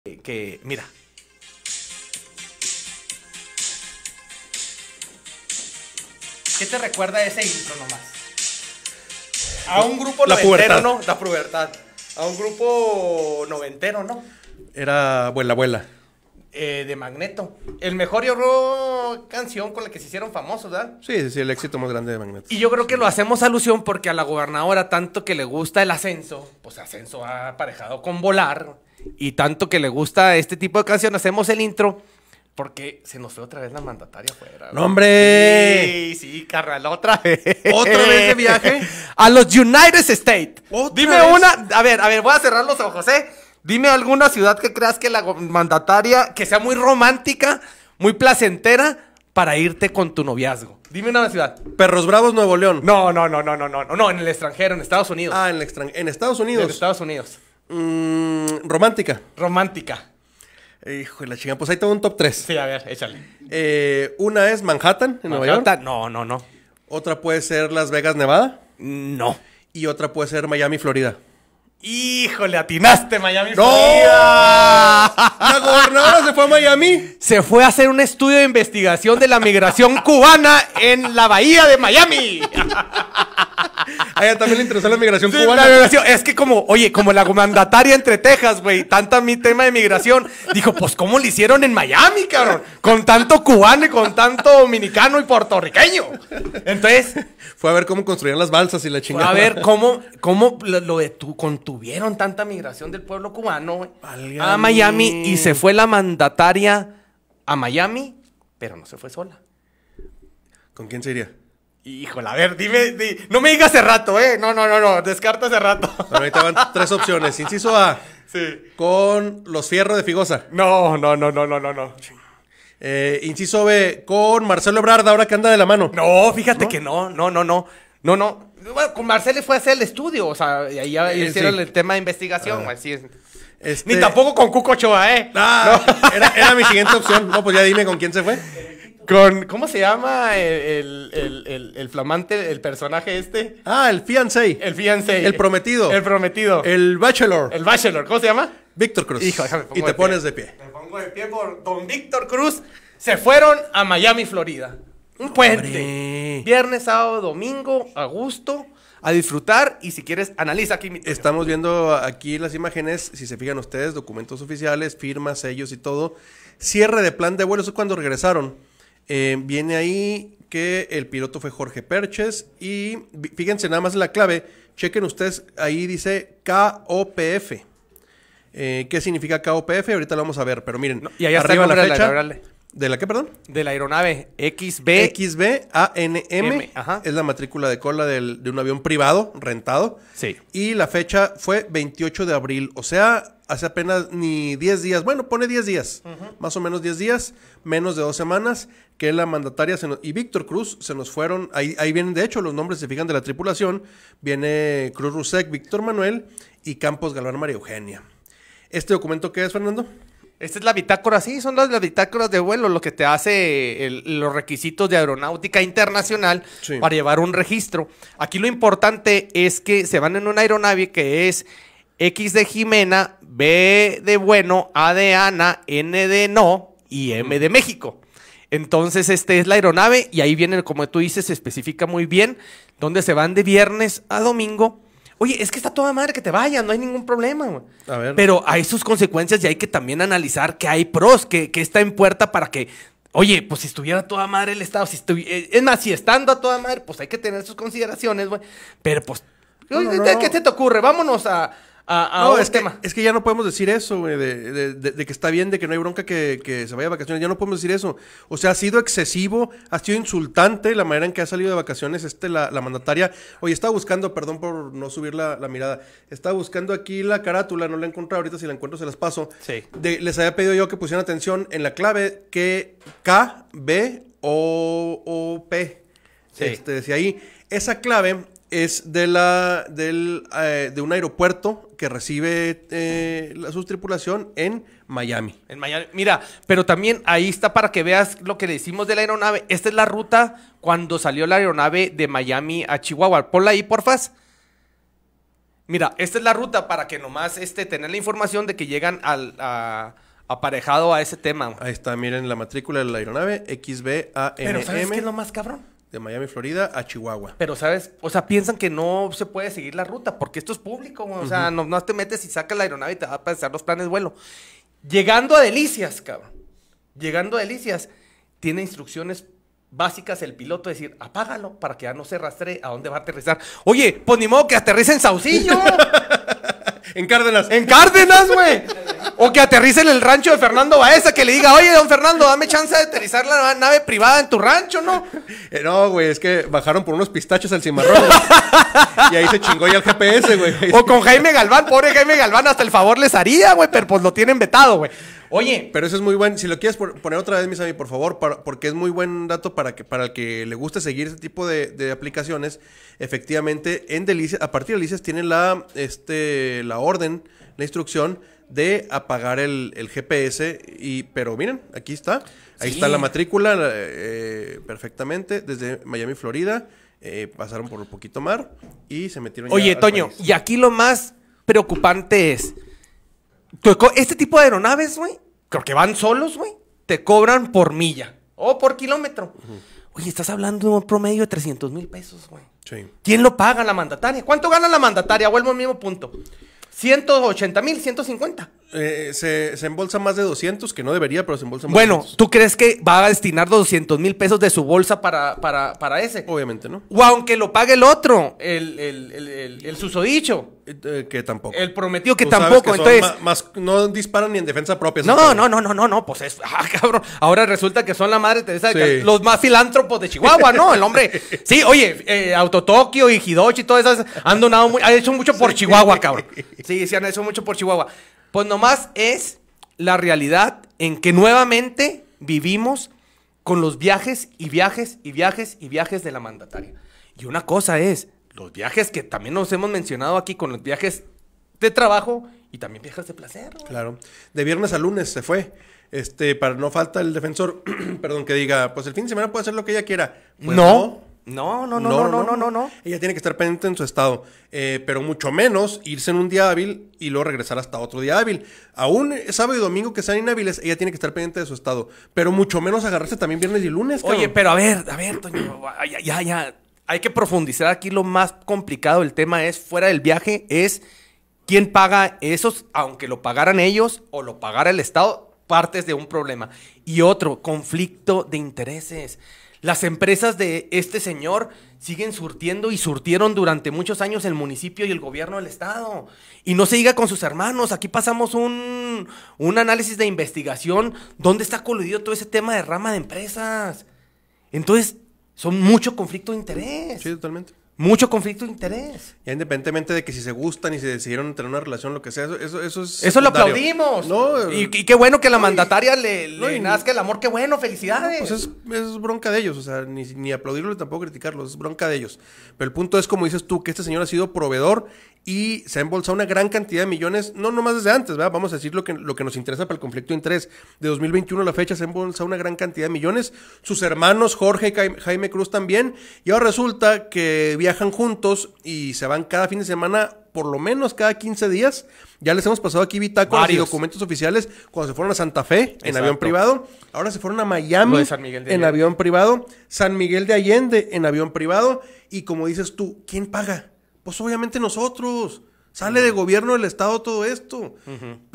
Maru Campos con el novio en Miami
Resulta que la gobernadora se nos fue oootra vez de viaje al extranjero, de viernes a lunes estuvo en las paradisiacas playas de Miami, brindando con su novio Víctor Cruz Russek, en avión privado, obviamente con todo pagado por gobierno del estado y de ello aquí les presentamos las pruebas. De esos 4 días ausente dos son hábiles, pero todos sabemos que incluso sábado y domingo un gobernante debe estar pendiente de la comunidad que gobierna, sobre todo cuando tienes un caos y verdadero desmadre en casa. Chihuahua se cae a pedazos! y maru sigue viviendo su faceta de gobernadora como si fuese toda una reina...